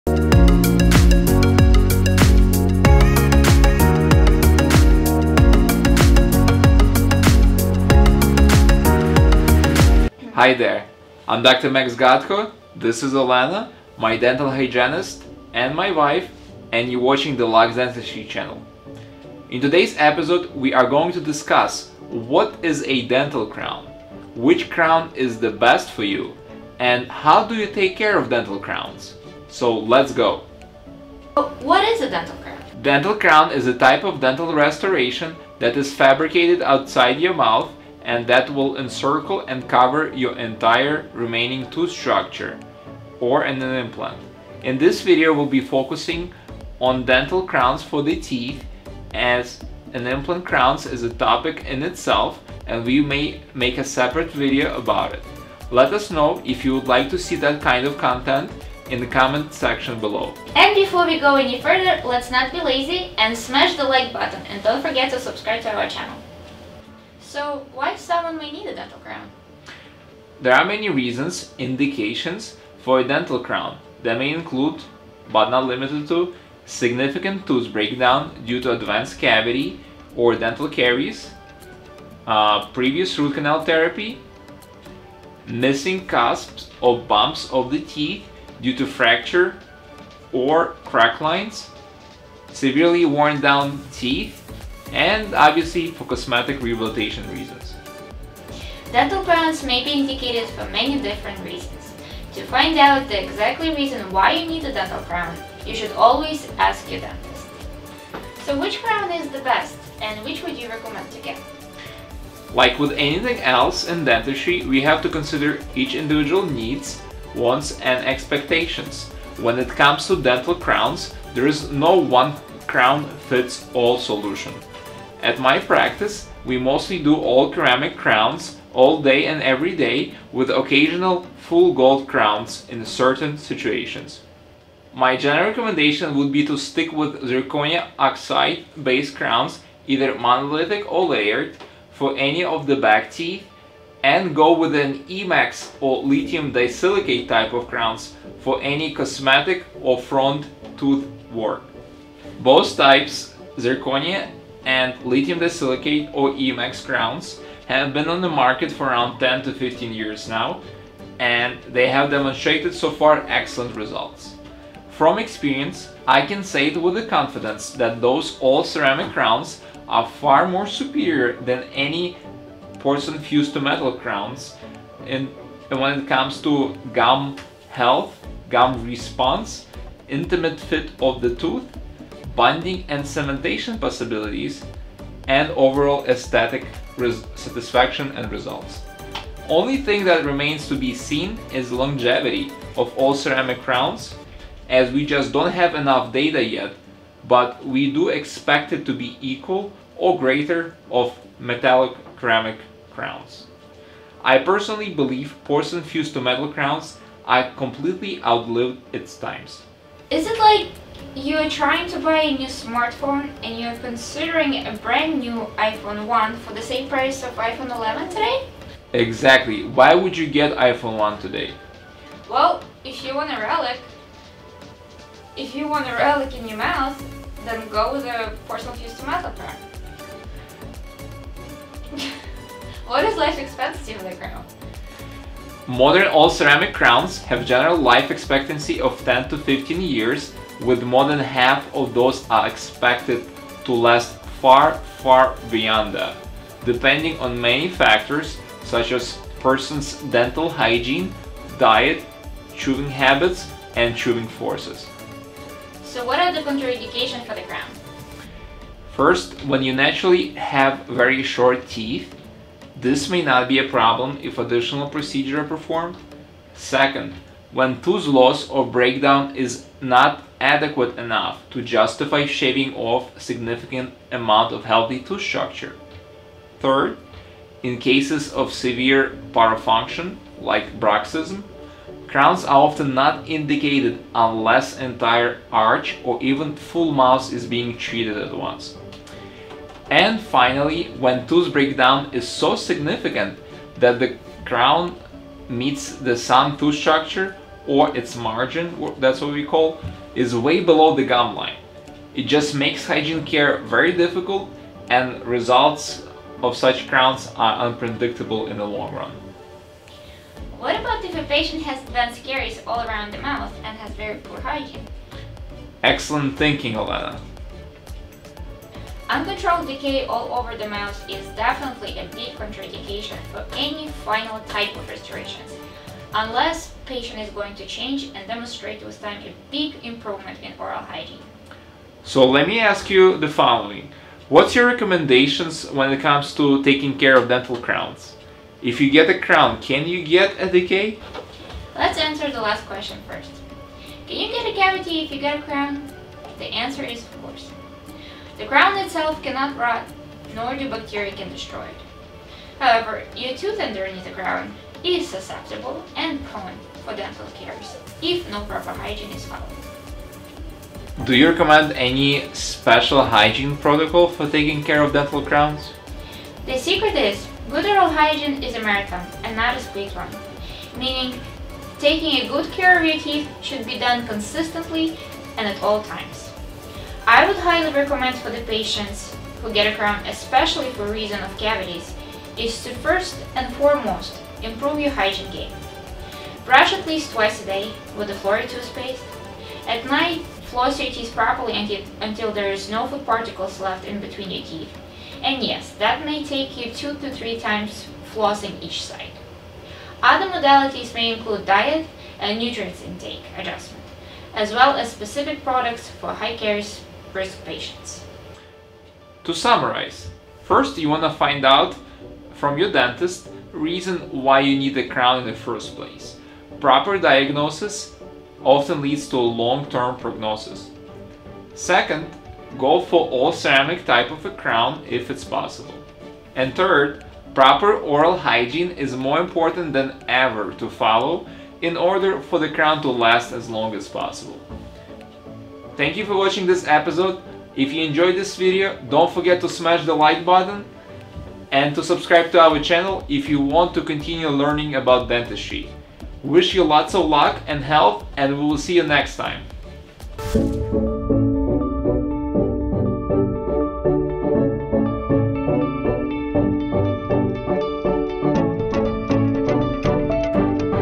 How to take care of dental crowns/ What dental crown to choose/ Indications for dental crowns
In this video, you will learn about dental crowns, what type of dental crowns we recommend, what are the indications and contraindications for dental crowns, and how do you take care of your new crown.

- to learn more or to become our patient.